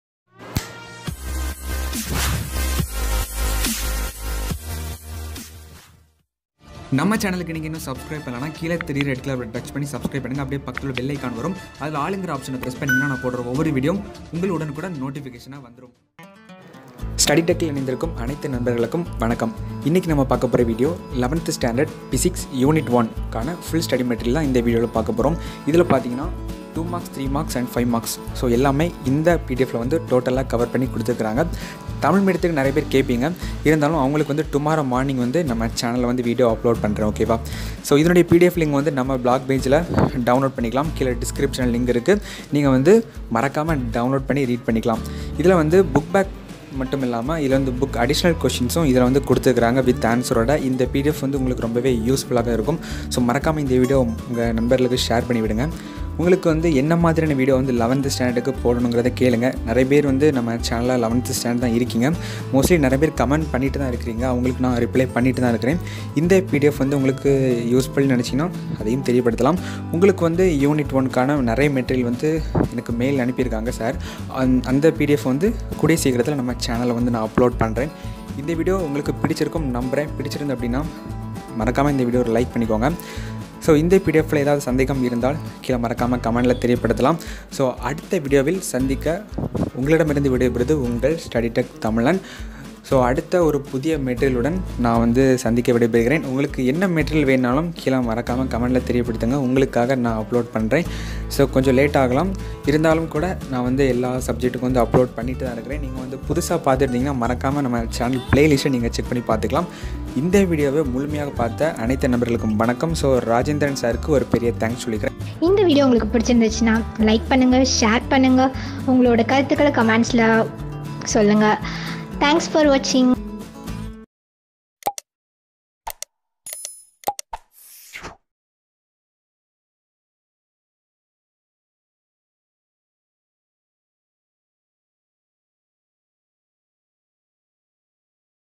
11th Std Physics | Chapter-1| Nature of Physical World And Measurement | English Medium | 2020-2021
Vidio Short Description :
* 11th Class Physics Volume 1 chapter-1 Full Study Materials featured in this video.

 * We will be uploading questions from the channel to the Fourteenth Class Mathematics course.

* Full Guide will be uploaded as soon as possible.

* Wish U All The Best For All frds 👍

📑 Pdf Title.   :  11th Std Physics Chapter-1 Full
                              Study Material

🗒 PDF Tipe     :   English Medium

🖋 PDf Preparation.  :
 Rajendran M
, M.Sc.,M.Ed.,C.C.A.,
P.G.ASSISTANT IN PHYSICS ,
SRMHSS,KAVERIYAMOONDI,
THIRUVANAMALAI.

🌍Social Profile 🌎

©️Study Tech Tamil